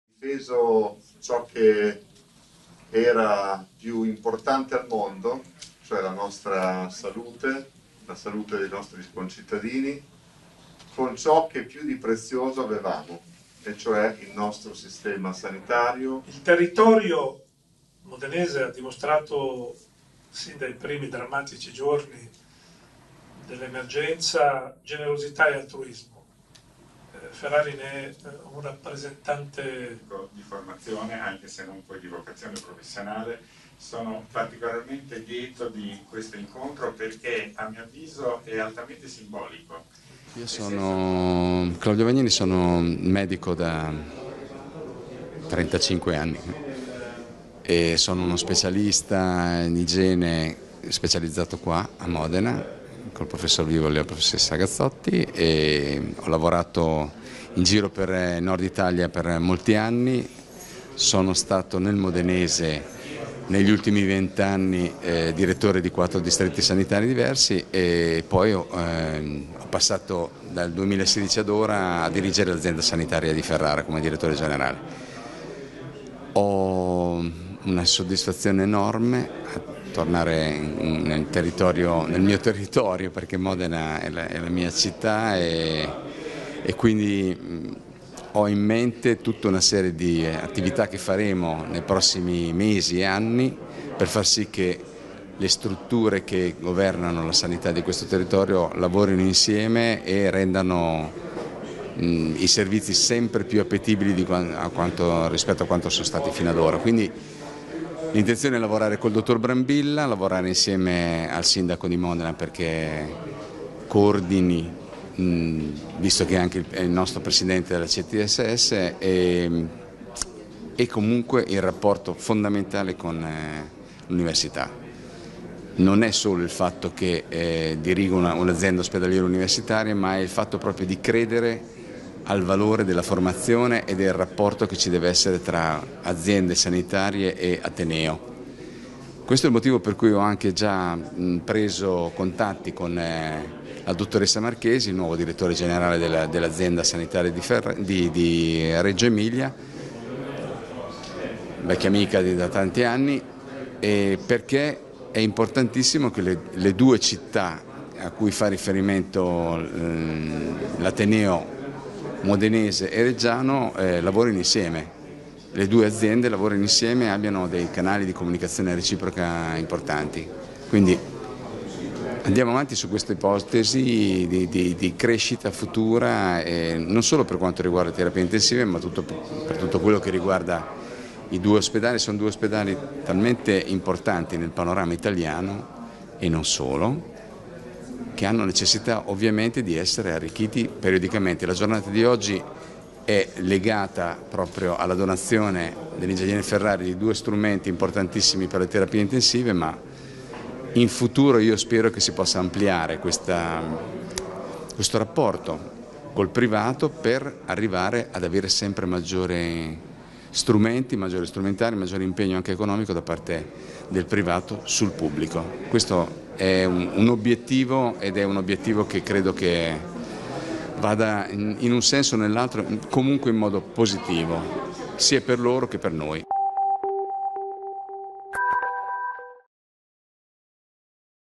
Intervista a Claudio Vagnini dirigente ospedali modenesi
Claudio Vagnini, nuovo direttore dell'Azienda ospedaliero universitaria, si presenta all'esordio dela guida dei due ospedali, Policlinico e Baggiovara.